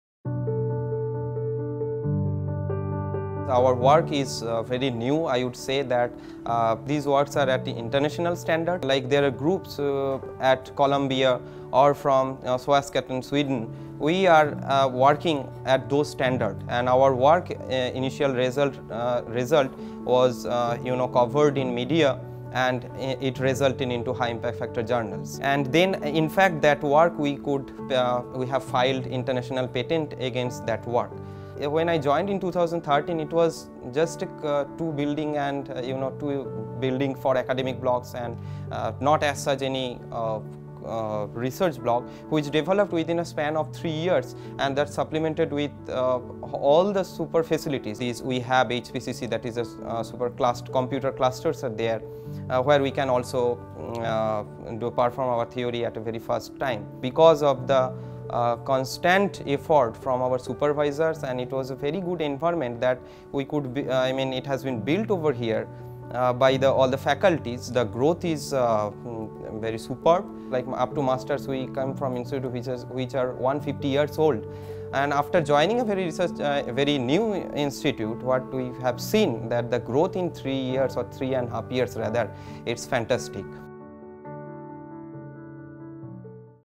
World-class research facilities at Shiv Nadar University
Mainak Banerjee, Final Year Ph.D. candidate at Department of Chemistry explains how the research facilities at Shiv Nadar University are at par with the top international universities across the world.